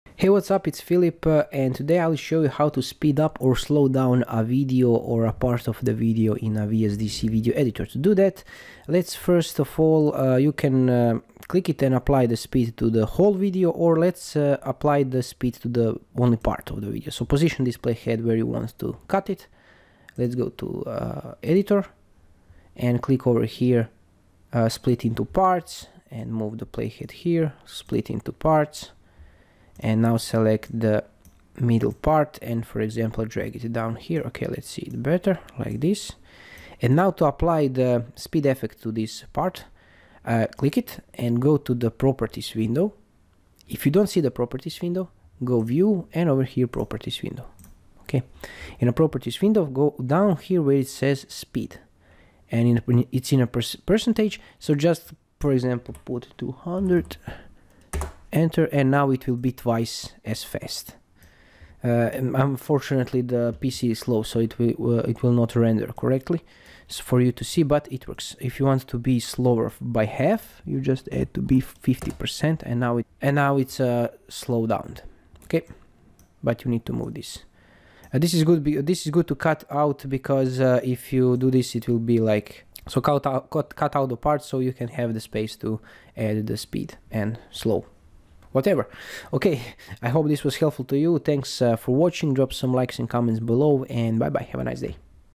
VSDC Free Video Editor: How To Speed Up Video In VSDC Video Editor😎
Quick video on how to speed up the video in vsdc video editor. Use this to speed up your footage or slow down for some awesome effects.